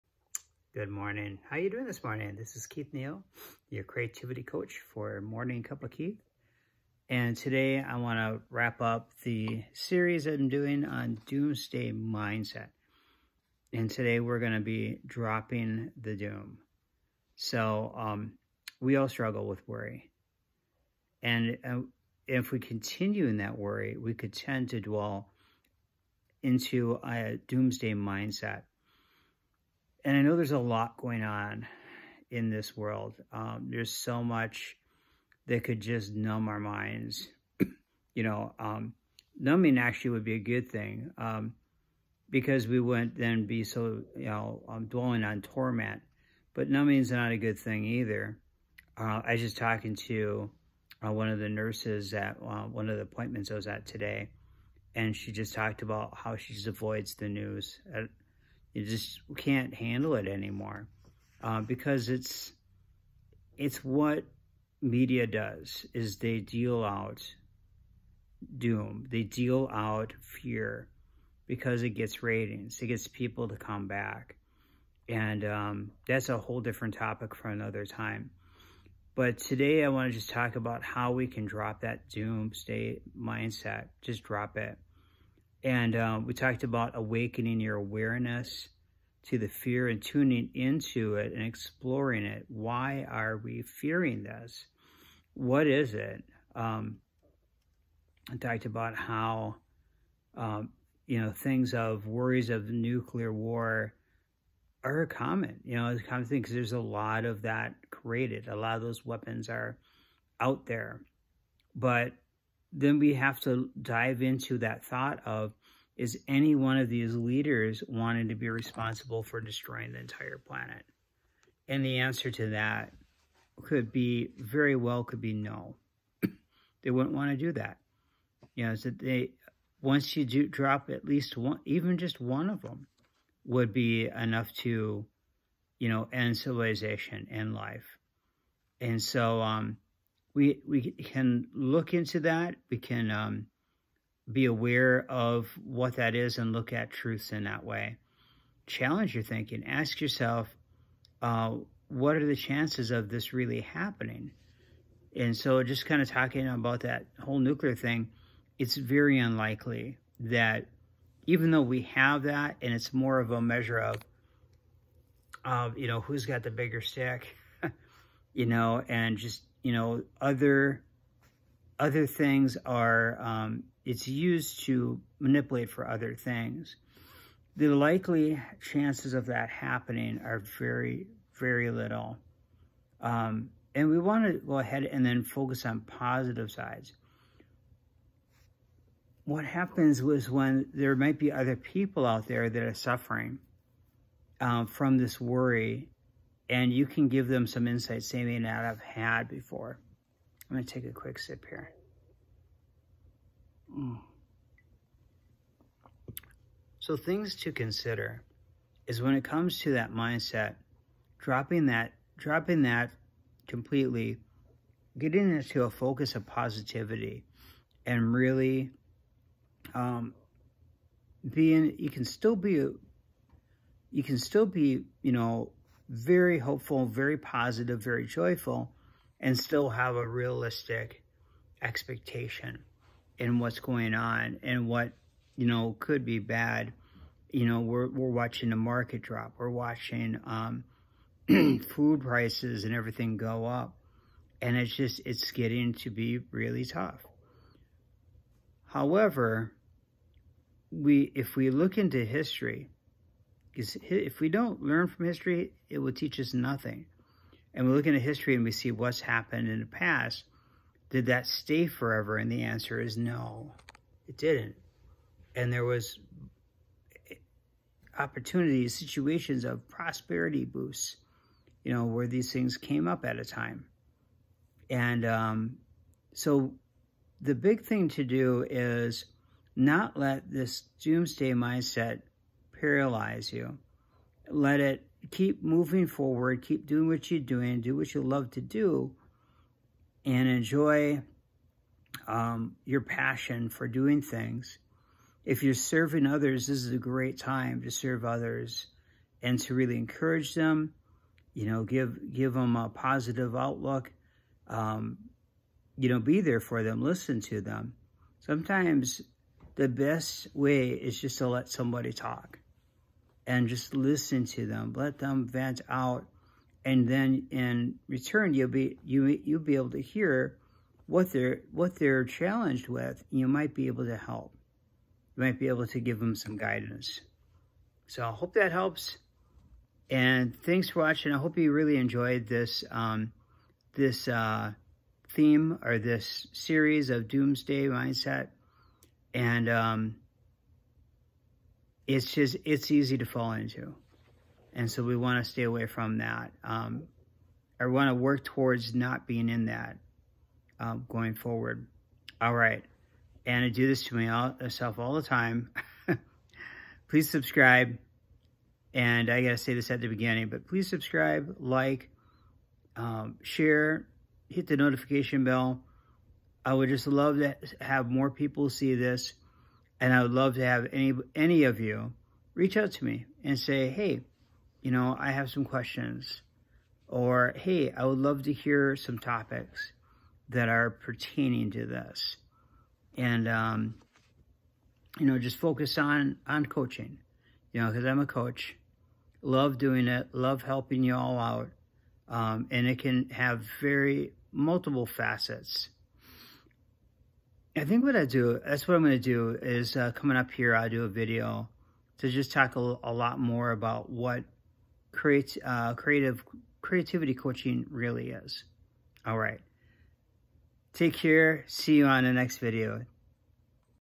Doomsday Mindset - Drop the Doom
We discussed Awakening your Awareness, Challenge your thinking and Focusing on the Positive in order to drop your doom.

In this video check out the details of this series wrap up.